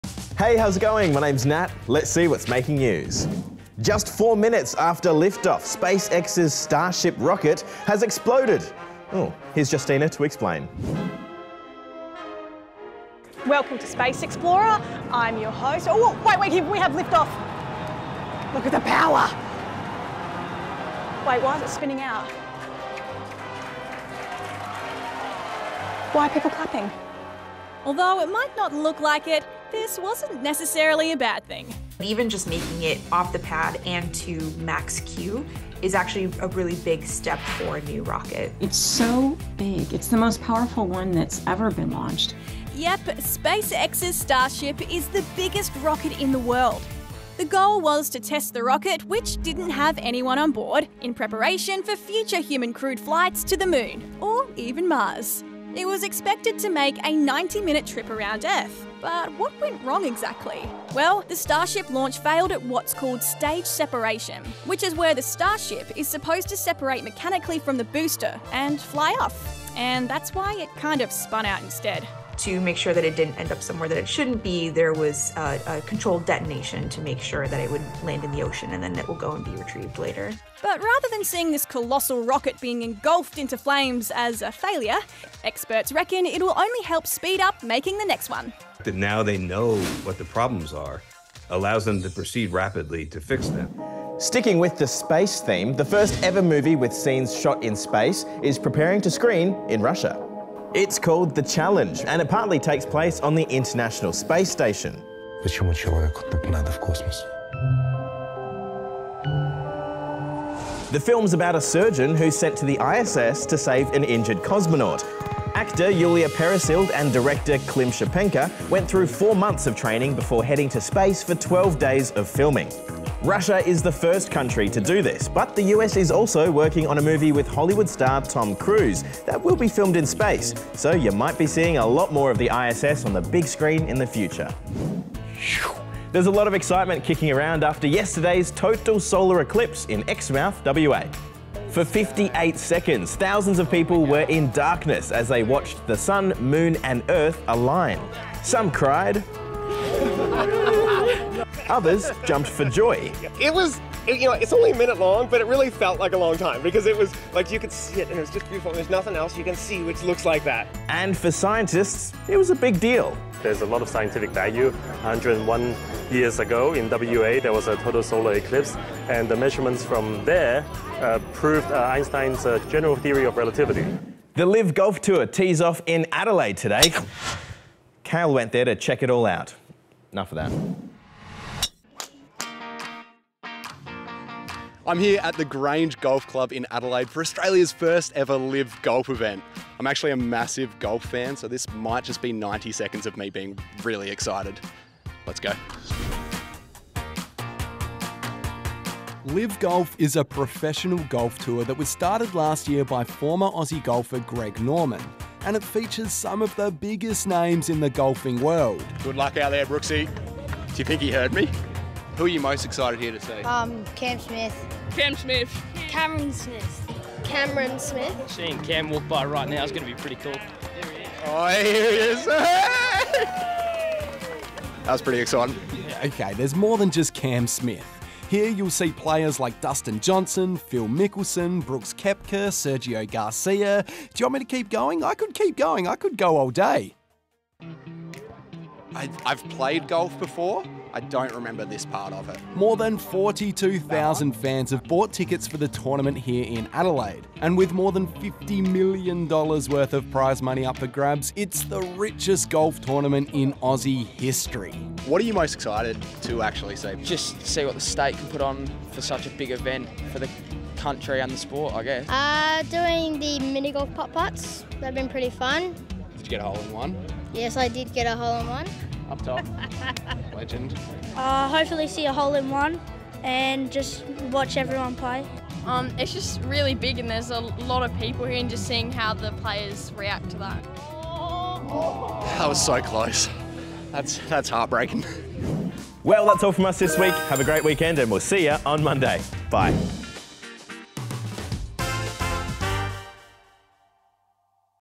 Why SpaceX's Starship Rocket Exploded
Just four minutes after lift-off, SpaceX's Starship rocket has exploded! Although it might not look like it, this was not necessarily a bad thing.

SPACEX STARSHIP EXPLODES
Yep, Space X's Starship is the biggest rocket in the world. The goal was to test the rocket, which didn't have anyone on board, in preparation for future human crewed flights to the moon or even Mars. It was expected to make a 90-minute trip around earth, but what went wrong exactly? Well, the Starship launch failed at what's called 'stage separation', which is where the starship is supposed to separate mechanically from the booster and fly off. And that's why it kind of spun out instead. But rather than seeing this colossal rocket being engulfed into flames as a failure, experts reckon it'll only help speed up making the next one.

SPACE MOVIE
Sticking with the space theme, the first-ever movie with scenes filmed in space is preparing to screen in Russia. It's called The Challenge and it partly takes place on the International Space Station. The film's about a surgeon who's sent to the ISS to save an injured cosmonaut. Actress Yulia Peresild and director Klim Shipenko went through four months of training before heading to space for 12 days of filming. Russia was the first country to do this, but the US is also working on a movie with Hollywood star Tom Cruise that will be filmed in space, so we might be seeing a lot of the ISS on the big screen in the future.

ECLIPSE REACTION
There's a lot of excitement kicking around after yesterday's total solar eclipse in Exmouth WA. For 58 seconds, thousands of people were in darkness as they watched the sun, moon, and Earth align. Some cried, others jumped for joy, and for scientists, it was a big deal.

LIV GOLF
The LIV Golf tour tees-off in Adelaide today! Cale went along to check it all out. LIV Golf is a professional golf tour that was started last year by former Aussie golfer Greg Norman, and it features some of the biggest names in the golfing world. Okay, there's more than just Cam Smith. Here you'll see players like Dustin Johnson, Phil Mickleson, Brooks Koepka, Sergio Garcia. Do you want me to keep going? I could definitely keep going. More than 42,000 fans have bought tickets for the tournament here in Adelaide where 12 teams of 4 players each will be battling it out for more than 30 million dollars worth of prize money.